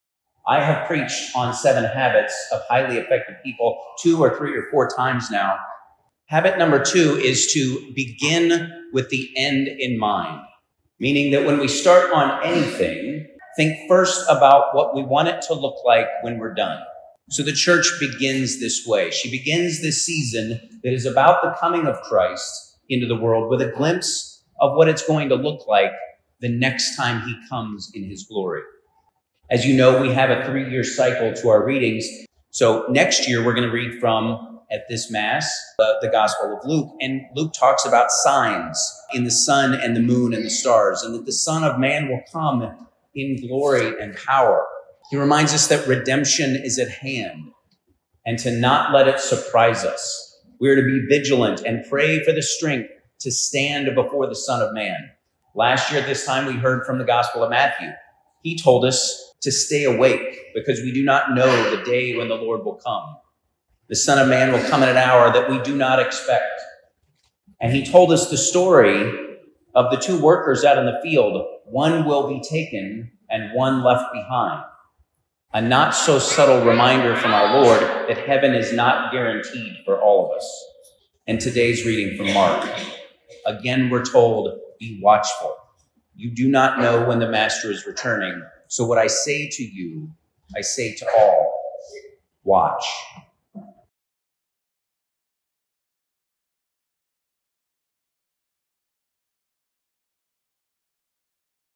Homily Recap - December 3, 2023 - First Sunday of Advent
Please watch a brief summary of Deacon Jason's homily from last weekend.